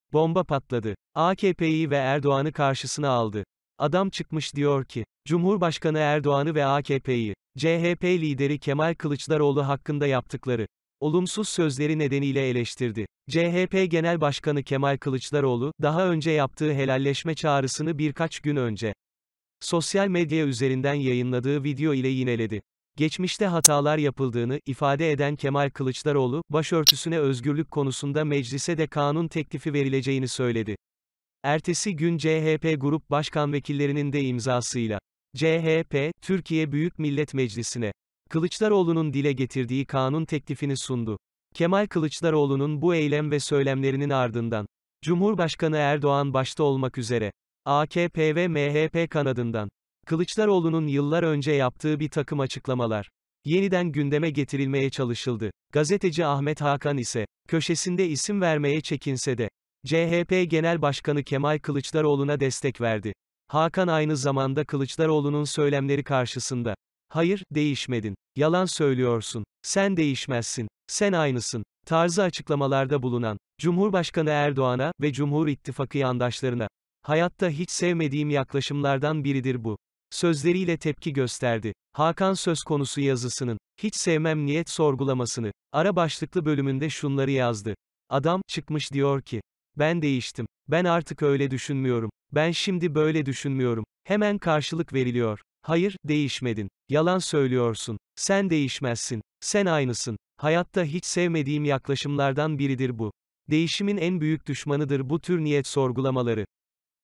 ANKARA KARIŞTI..ERDOĞAN VE AKP'Yİ ÇILDIRTDI..''YALAN SÖYLÜYORSUN''
Geçmişte hatalar yapıldığını ifade eden Kemal Kılıçdaroğlu başörtüsüne özgürlük konusunda Meclis’e de kanun teklifi verileceğini söyledi.